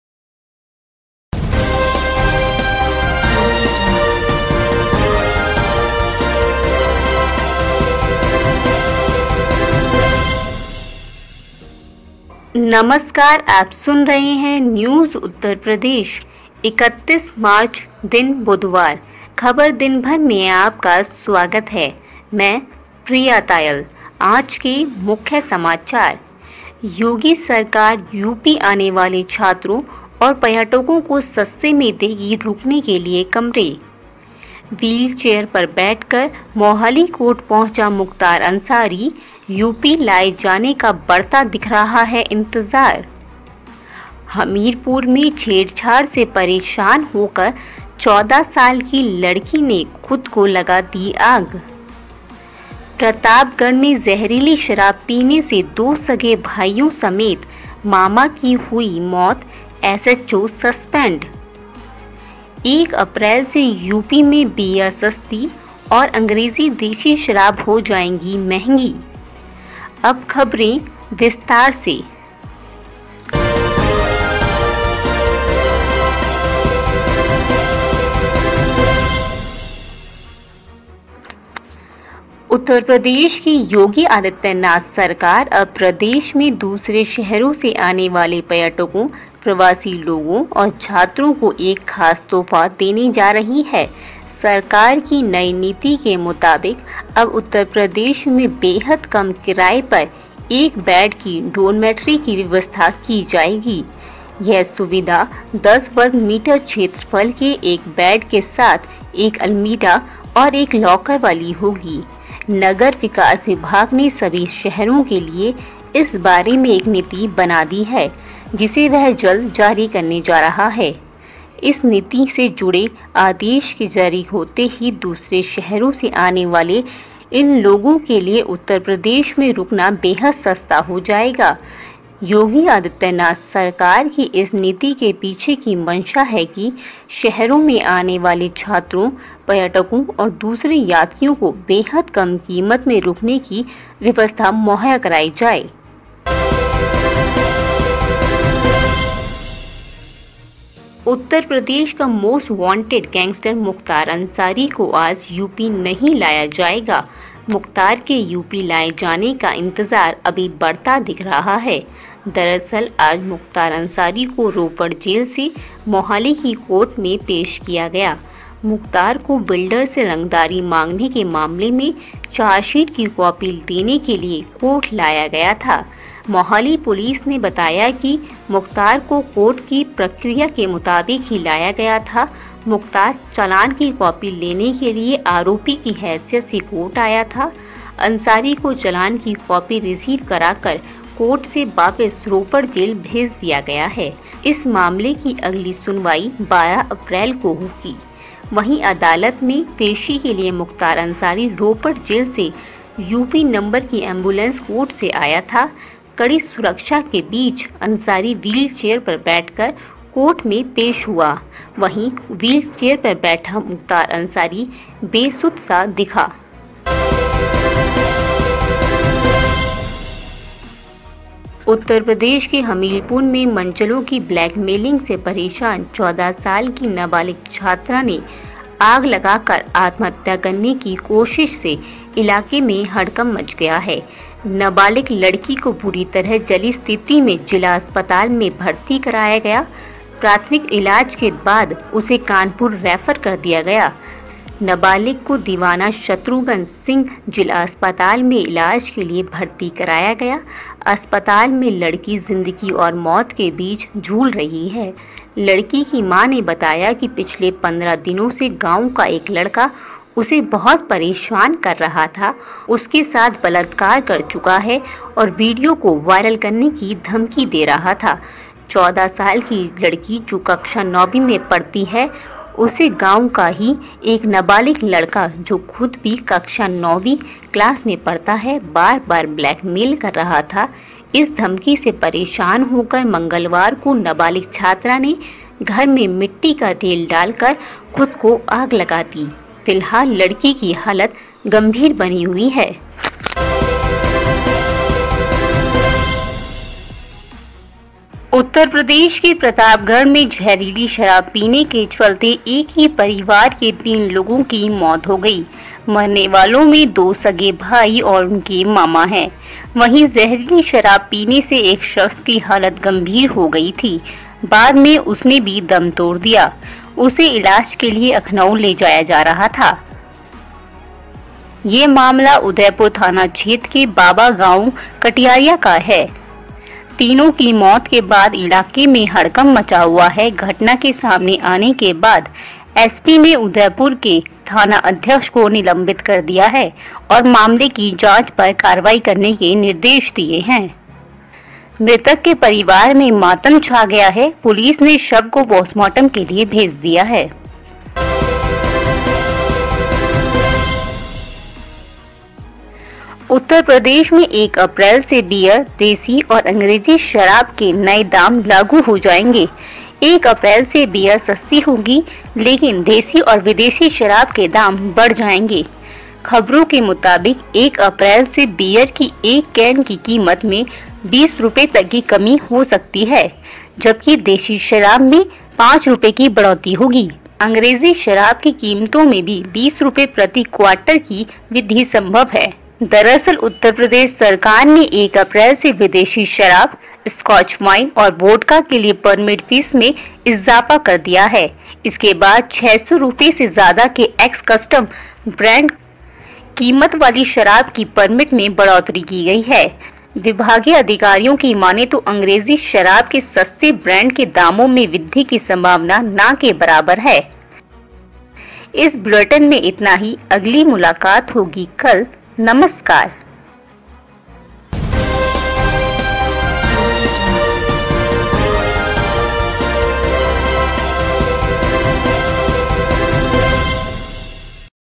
NYOOOZ Uttar Pradesh का Podcast 'ख़बर दिनभर',सुनिए आज की 5 सबसे बड़ी ख़बरें। 31 March| News Bulletin
1. योगी सरकार यूपी आने वाले छात्रों और पर्यटकों को सस्ते में देगी रुकने के लिए कमरे
2. व्हील चेयर पर बैठकर मोहाली कोर्ट पहुंचा मुख्तार अंसारी, यूपी लाए जाने का बढ़ता दिख रहा है इंतजार
3. हमीरपुर में छेड़छाड़ से परेशान होकर 14 साल की लड़की ने खुद को लगा ली आग
4. प्रतापगढ़ में जहरीली शराब पीने से दो सगे भाइयों समेत मामा की हुई मौत, SHO सस्पेंड
5. 1 अप्रैल से यूपी में बीयर सस्ती और अंग्रेजी-देसी शराब हो जाएगी महंगी